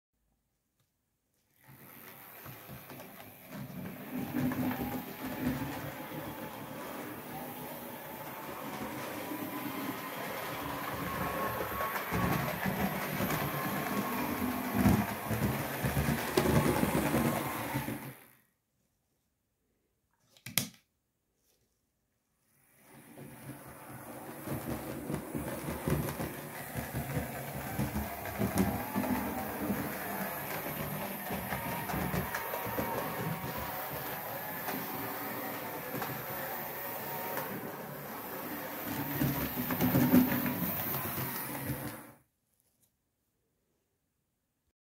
OO Gauge Oxford Rail Adams Radial 4-4-2 BR Early Black ‘30584’.
OO Gauge Oxford Rail Adams Radial 4-4-2 BR Early Black ‘30584’ running on my layout.